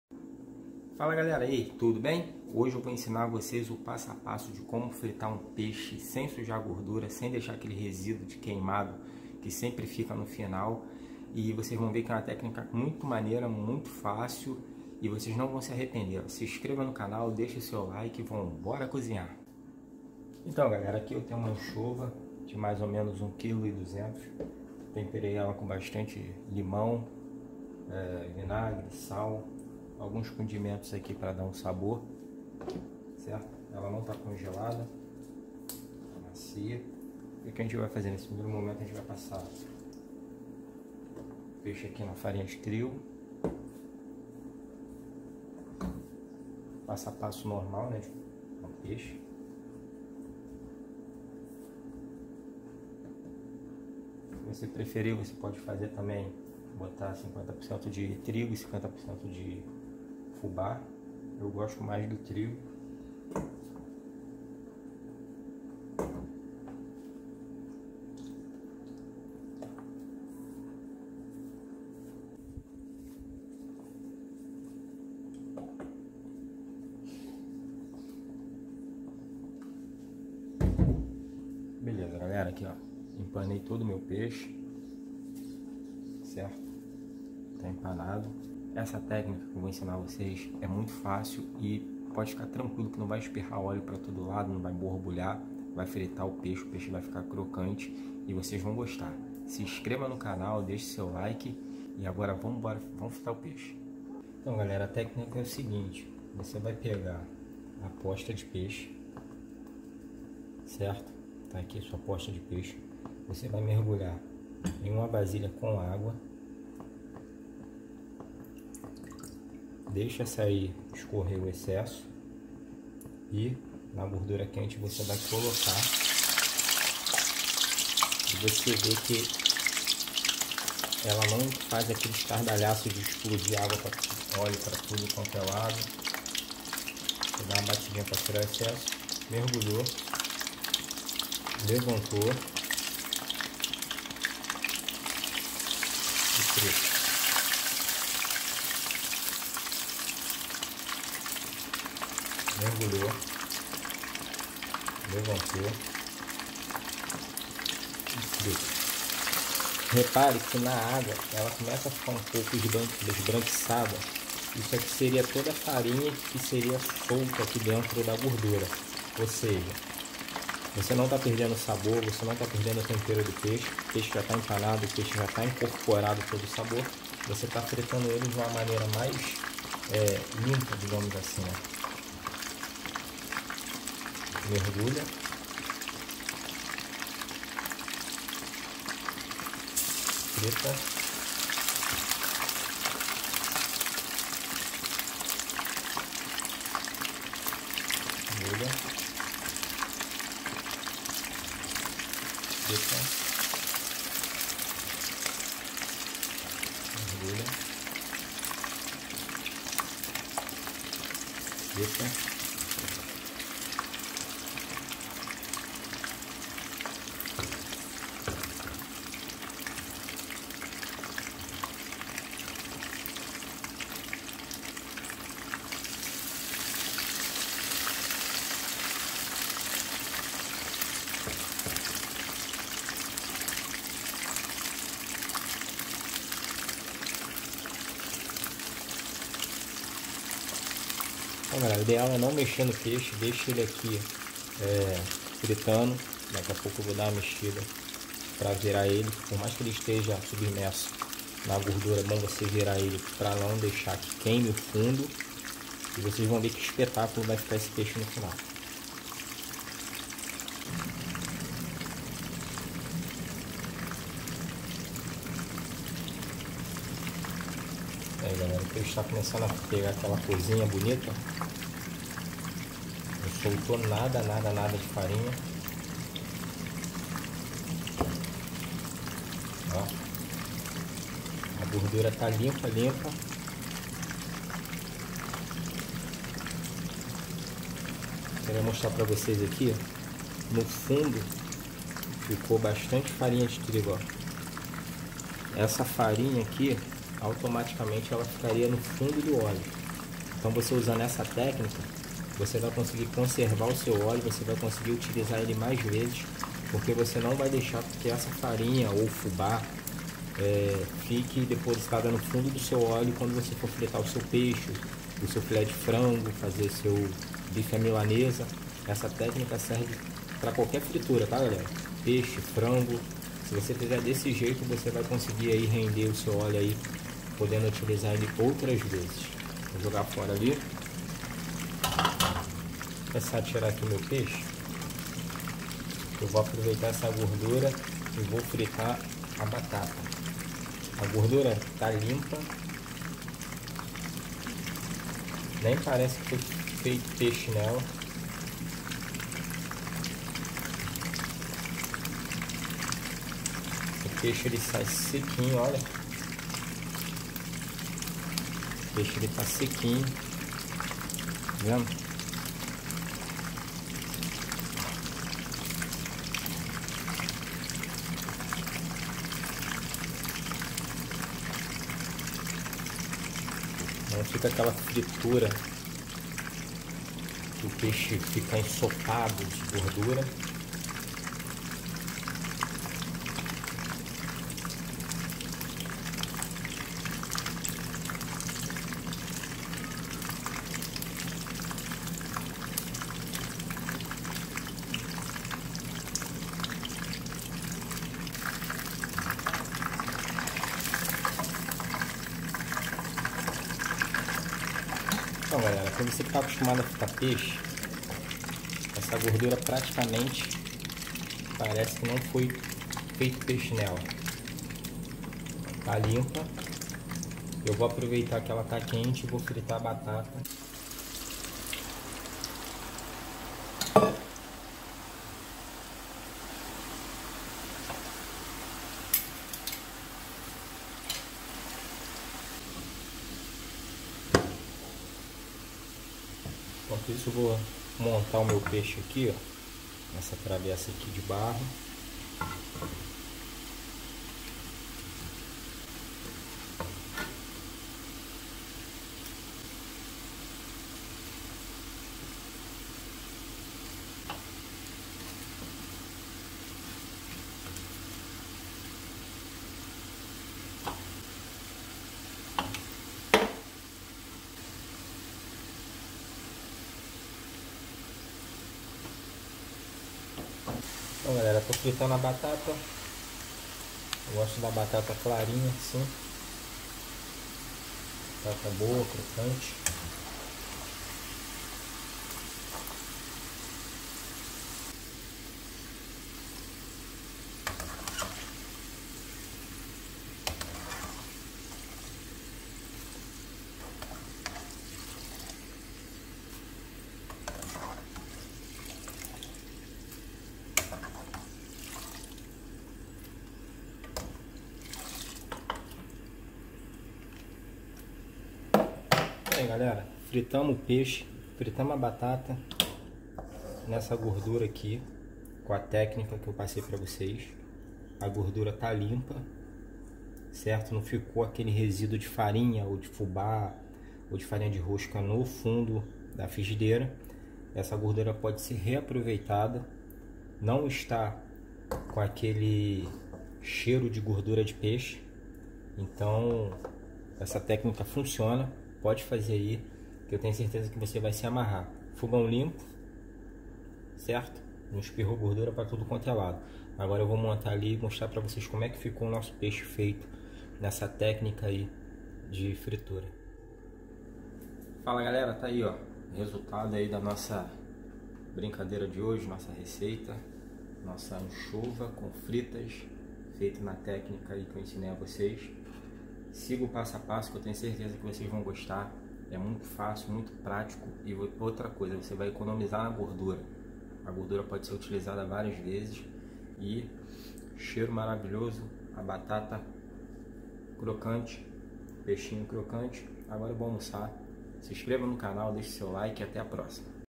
Como Frita Peixe sem Queimar o Óleo
Quem já se arriscou a fazer um peixinho frito na cozinha, já percebeu que, após algumas frituras, o óleo acaba queimando e soltando um sabor amargo. Na realidade, esse gostinho amargo e o fato do óleo escurecer vem do excesso de farinha de trigo usado para empanar o alimento. Quando a farinha se solta do filé, ela ficará imersa na gordura quente e, com o tempo, acaba queimando, escurecendo o óleo e dando um sabor desagradável à comida. Mas agora podemos evitar isso.